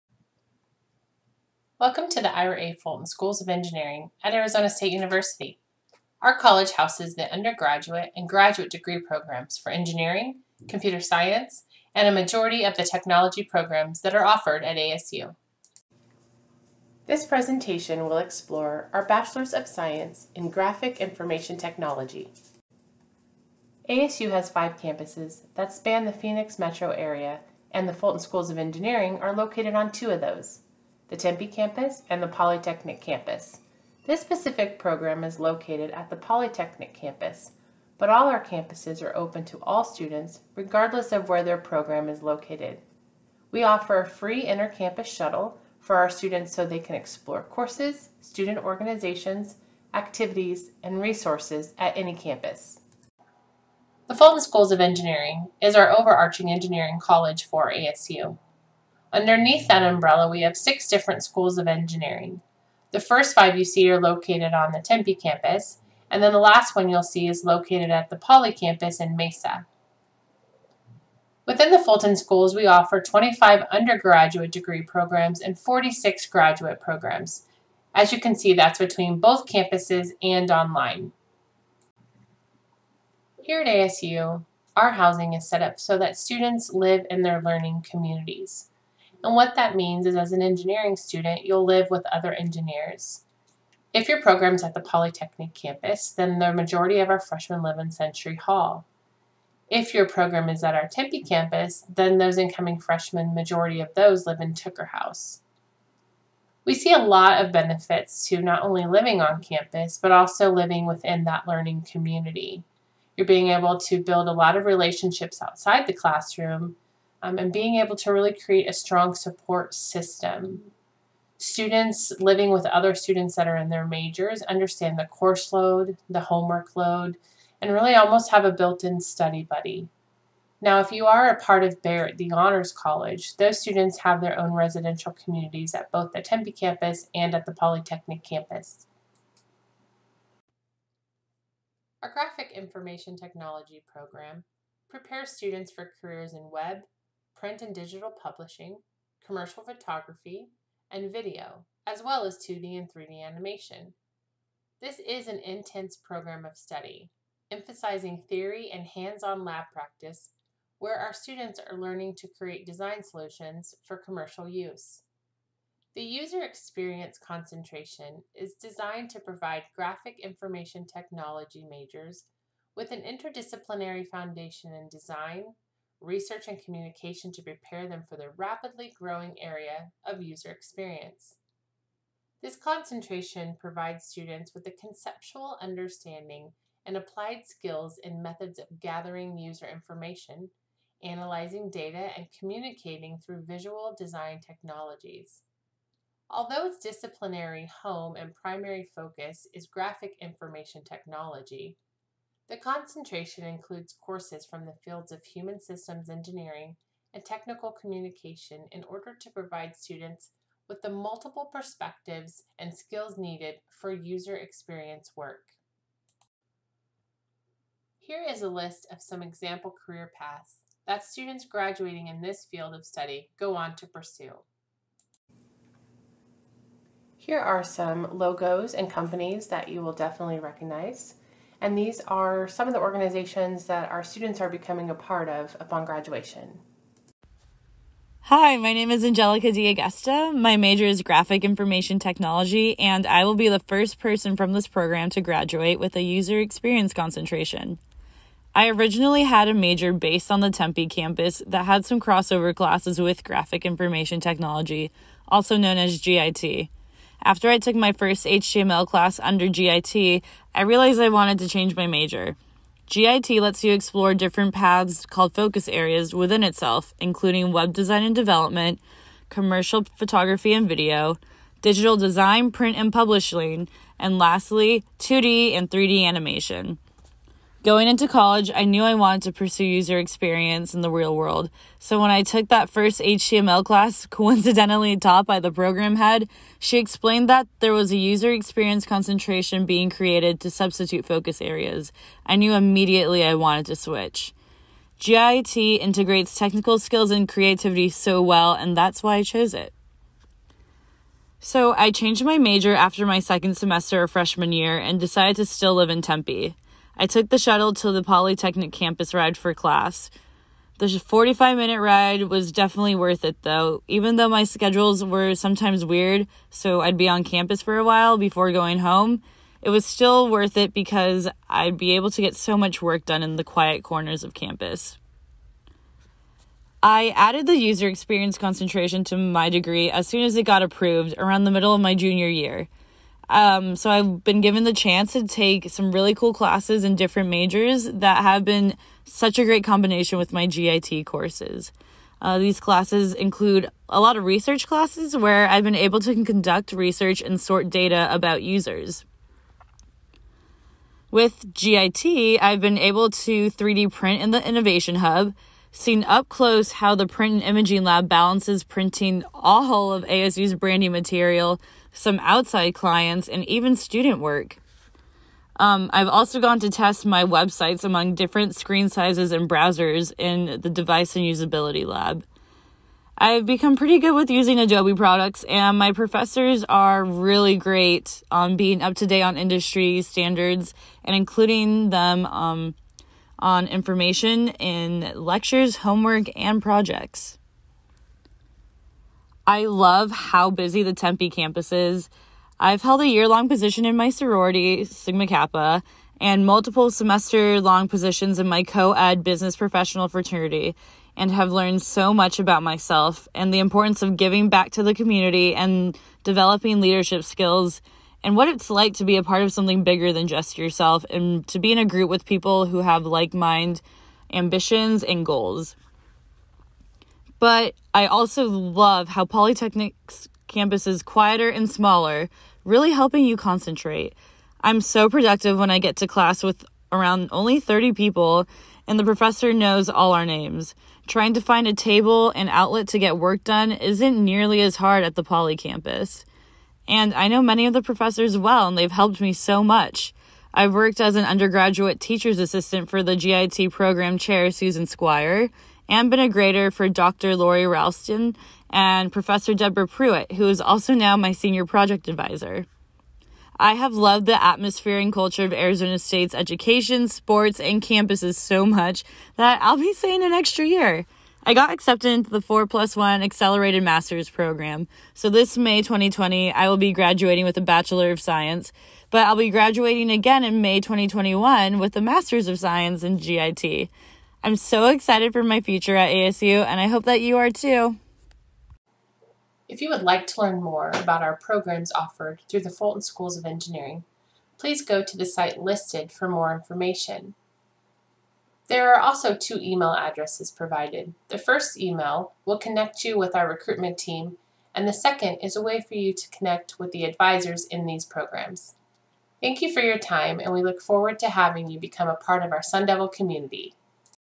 Fulton Schools Degree Webinar: BS Graphic Information Technology (User Experience)
This video will explore the graphic information technology degree program, user experience concentration, housed at ASU's Polytechnic Campus.